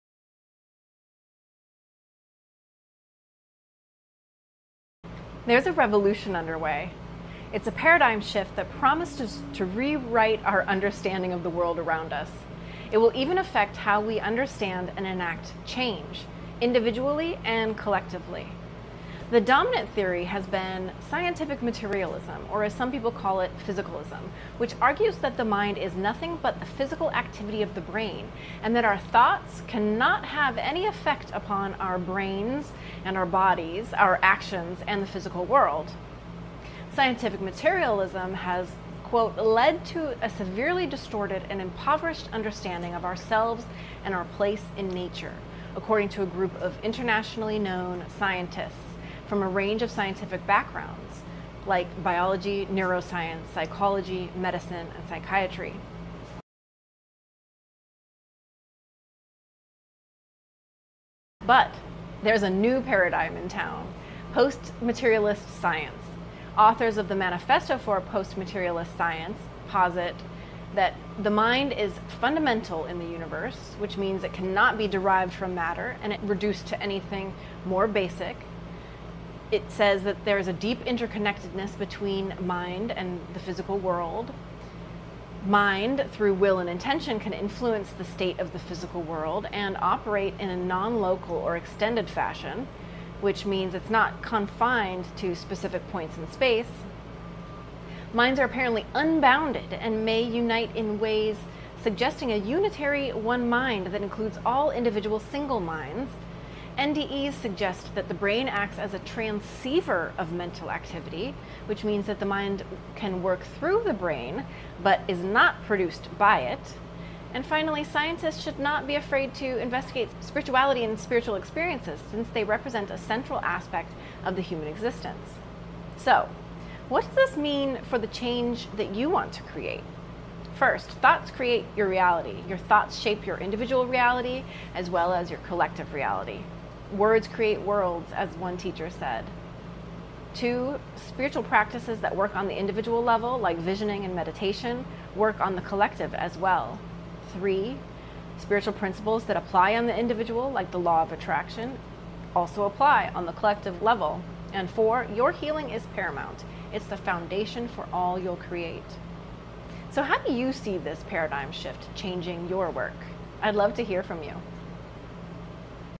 Science Is Finally Catching Up…Here’s How
For centuries, materialist science has told us that our minds are nothing more than the product of our brains, that consciousness is an accidental byproduct of matter.

But now, a quiet revolution is underway. Post-materialist science is emerging, and with it, a profound recognition:

“The shift from materialist science to post-materialist science may be of vital importance to the evolution of the human civilization. It may be even more pivotal than the transition from geocentrism to heliocentrism.”
– Authors of the Manifesto for a Post-Materialist Science

What does this mean for you?

It means your mind isn’t just a passenger in your life – it’s a creator. Your thoughts and intentions shape your reality and ripple out to shape our collective future.

In this video, I share how this paradigm shift changes everything we know about ourselves, our healing, and the change we’re here to create.

✨ How does this shift change your work, your vision, your life? Drop a comment. I’d love to hear.